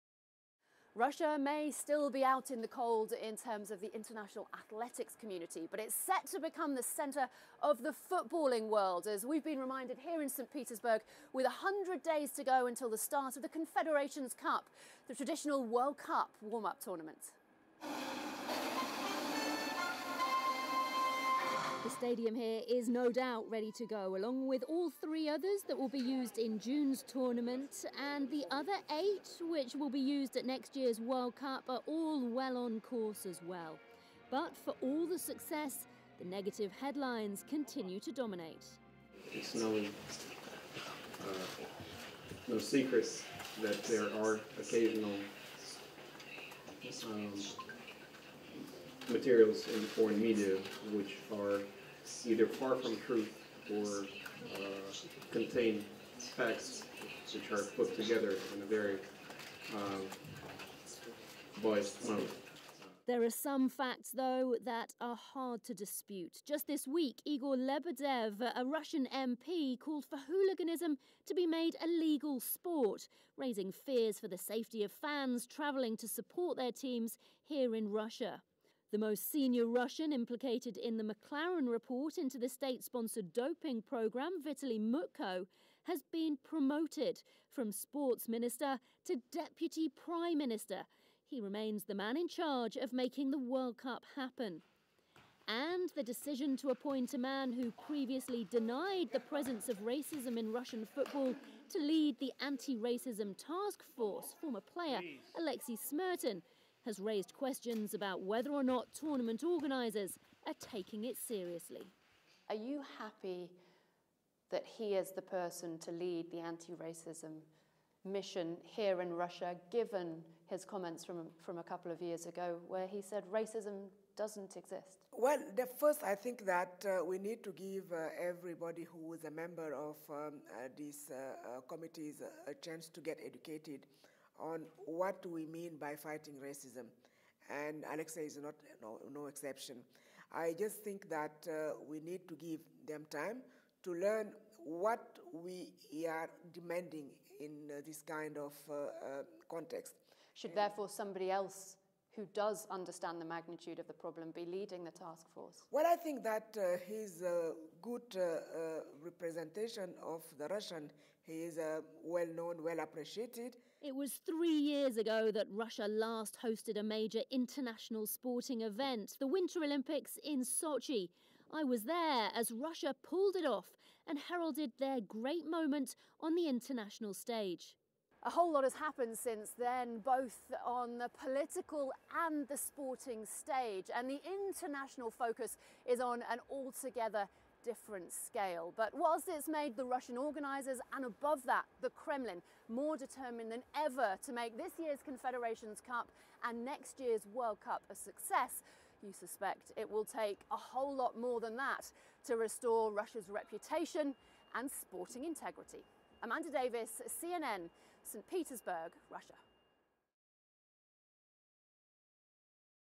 Is Russia ready to host the World Cup?
Can Russia convince the doubters it will deliver a successful year of football? CNN's Amanda Davies reports from St. Petersburg, Russia.